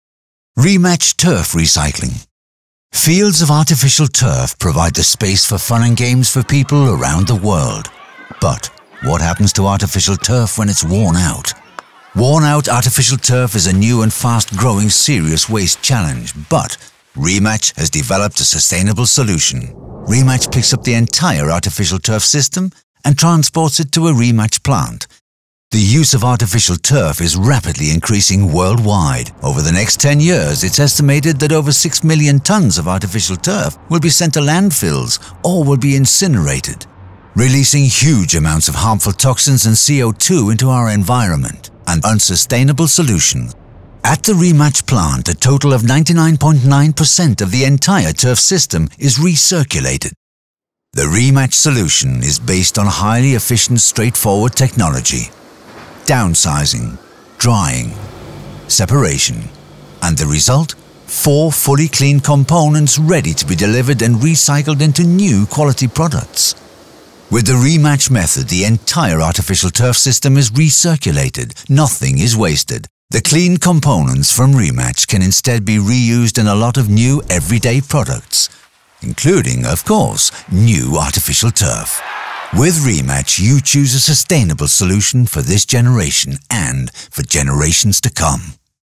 Re-Match - Brand New Method of Turf Recycling
Re-Match has developed a method to recycle all components in used artificial turf